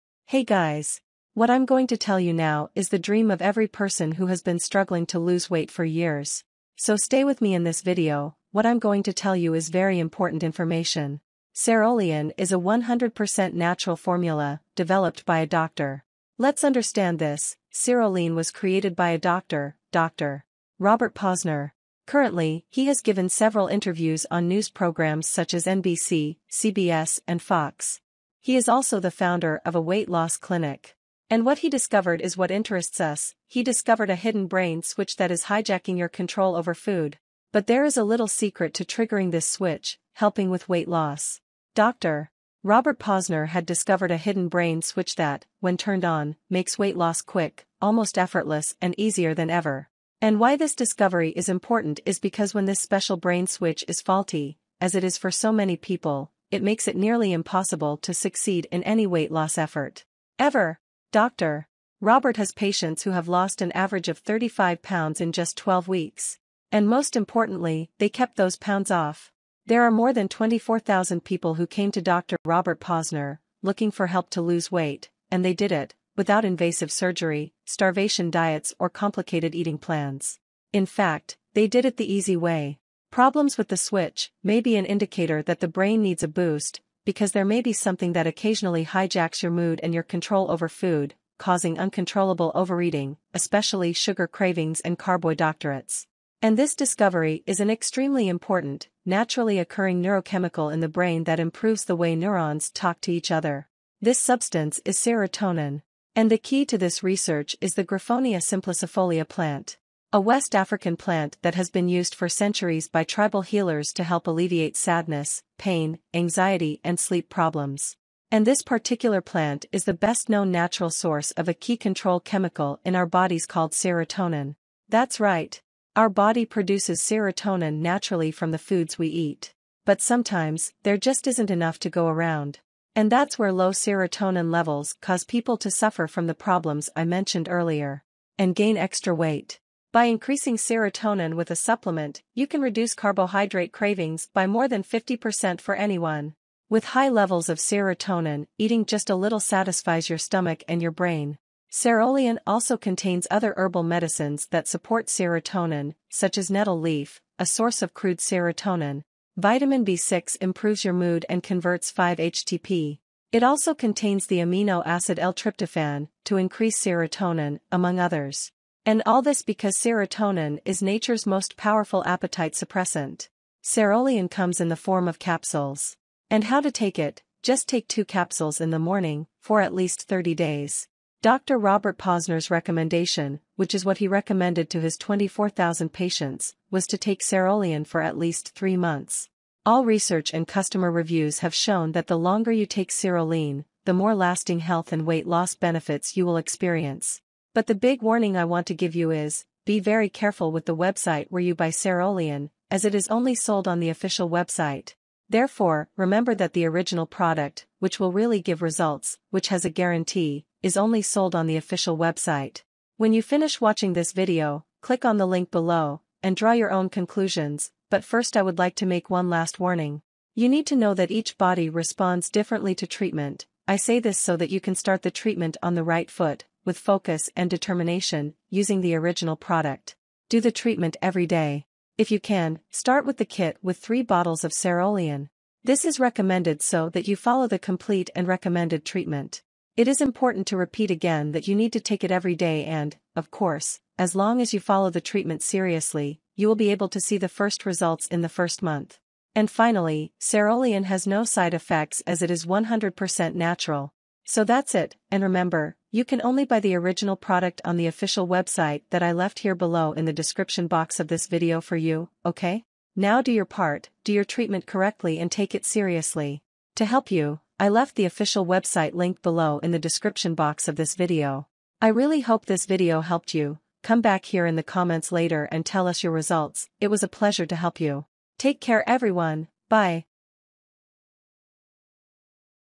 SEROLEAN REVIEW (⛔THE TRUTH!⛔🔥) SEROLEAN REVIEWS - SeroLean Weight Loss Supplement
Serolean is a 100% natural formula, developed by a doctor.
Let's understand this: SeroLean was created by a doctor, Doctor. Robert Posner.

Currently, he has given several interviews on news programs such as NBC, CBS and FOX.

He is also the founder of a weight loss clinic.
And what he discovered is what interests us: he discovered a Hidden 'Brain Switch' That Is Hijacking Your Control Over Food!
But there is a little secret to triggering this 'switch', helping with weight loss.

Doctor. Robert Posner had discovered a hidden 'brain switch' that, when 'turned on', makes weight loss quick, almost effortless and easier than ever.

And why this discovery is important is because when this special brain switch is faulty, as it is for so many people, it makes it nearly impossible to succeed in any weight loss effort. Ever!

Doctor. Robert has patients who have lost an average of 35 pounds in just 12 weeks! And Most Importantly, They Kept Those Pounds Off!
There are more than 24,000 people who came to Doctor Robert Posner, looking for help to lose weight... and they did it... without invasive surgery, starvation diets or complicated eating plans.

In fact, they did it the easy way...
Problems with the “switch”, may be an indicator that the brain needs a boost, because there may be something that occasionally hijacks your mood and your control over food...causing uncontrollable overeating...especially sugar cravings and carboiDoctorates.
And this discovery is an extremely important, naturally occurring neurochemical in the brain that improves the way neurons “talk to each other”…
This substance is SEROTONIN
And the key to this research is The Griffonia Simplicifolia Plant.

A West African plant that has been used for centuries by tribal healers to help alleviate sadness, pain, anxiety and sleep problems. And this particular plant is the best-known natural source of a key control chemical in our bodies called serotonin.

That's right! Our body produces serotonin naturally from the foods we eat. But sometimes, there just isn't enough to go around. And that's where low serotonin levels cause people to suffer from the problems I mentioned earlier... And gain extra weight.

By Increasing Serotonin With A Supplement, You Can Reduce Carbohydrate Cravings By More Than 50% For Anyone!
With HIGH Levels of Serotonin, Eating Just a Little Satisfies Your Stomach and Your Brain

Serolean also contains other herbal medicines that support serotonin, such as nettle leaf, a source of crude serotonin,

Vitamin B-6 improves your mood and converts 5-HTP;

It also contains the amino acid L-tryptophan, to increase serotonin, among others

And all this because Serotonin Is Nature's Most Powerful Appetite Suppressant!

Serolean comes in the form of capsules.
And how to take it: just take two capsules in the morning, for at least 30 days. Doctor Robert Posner's recommendation, which is what he recommended to his 24,000 patients, was to take Serolean for at least 3 months.

All research and customer reviews have shown that the longer you take SeroLean, the more lasting health and weight loss benefits you will experience.

00:00 – SEROLEAN
01:00 – What is SEROLEAN0
02:00 – How does SEROLEAN Work?
03:00 – SEROLEAN Benefits
04:20 – SEROLEAN Official Website